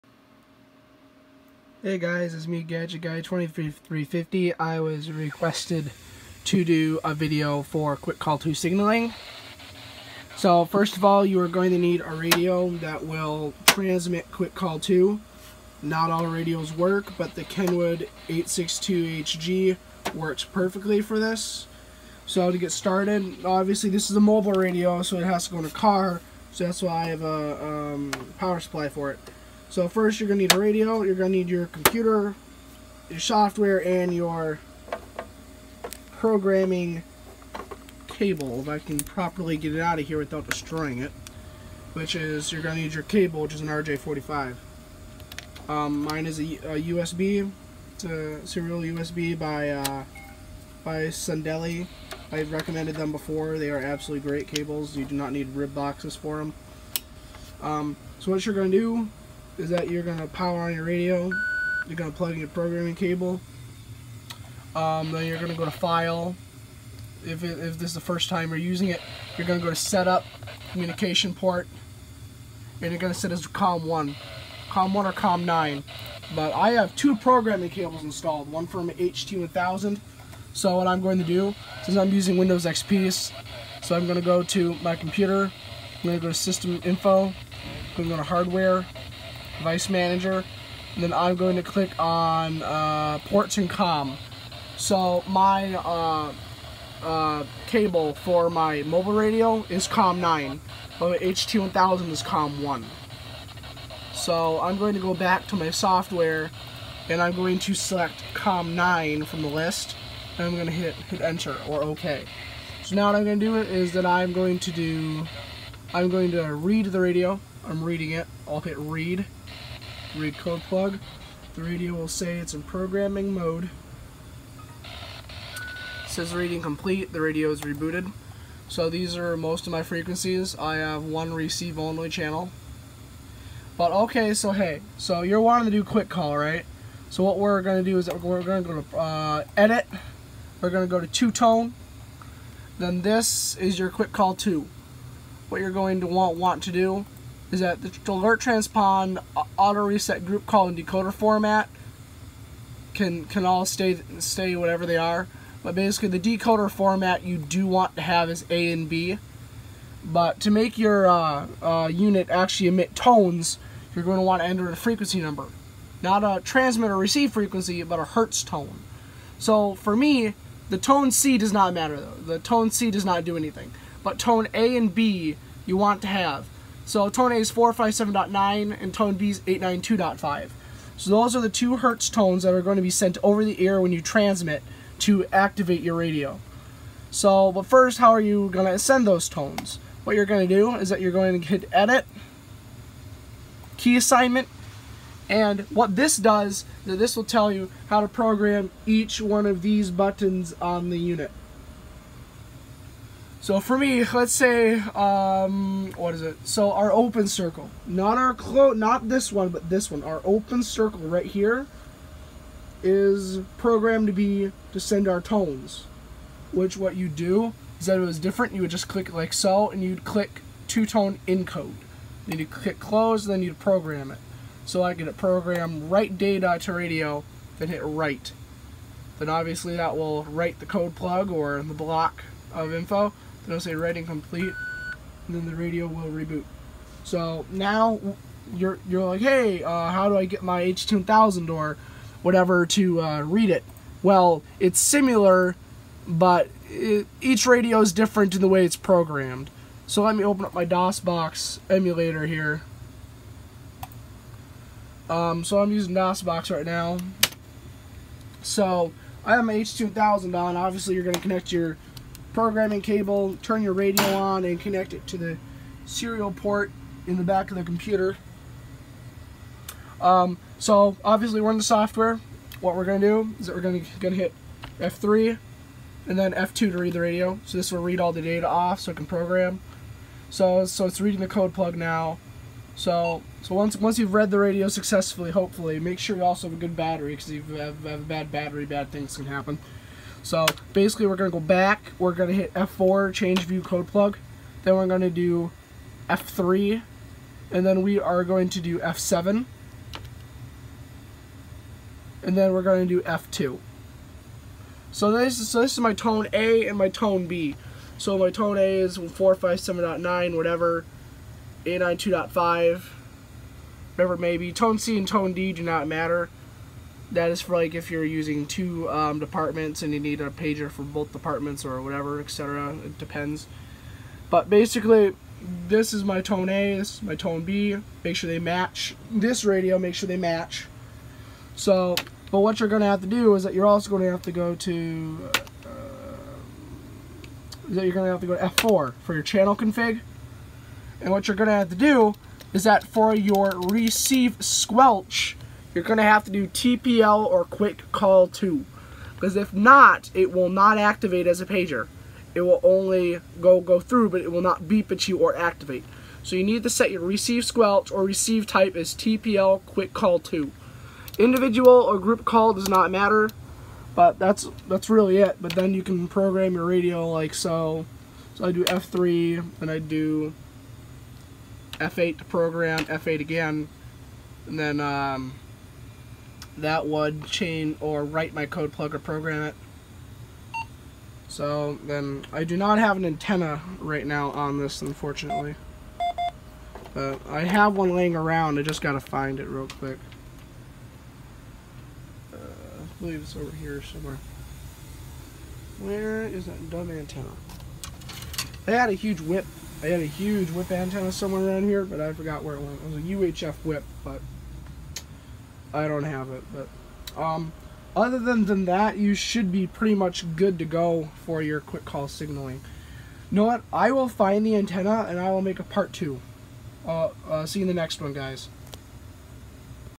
In-depth video on how to program quick call II signaling part1
This is an in-depth tutorial on how to program quick call II